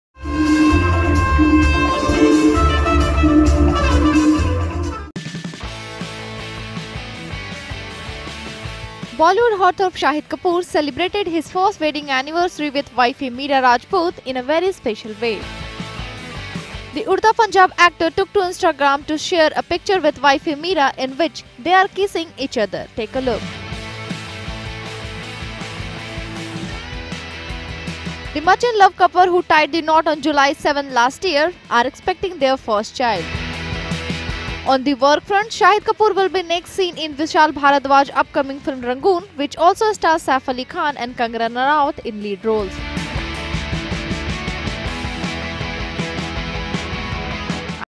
Shahid liplock with wifey Mira on first wedding anniversary
Bollywood heartthrob Shahid Kapoor celebrated his first wedding anniversary with wife Mira Rajput in a very special way.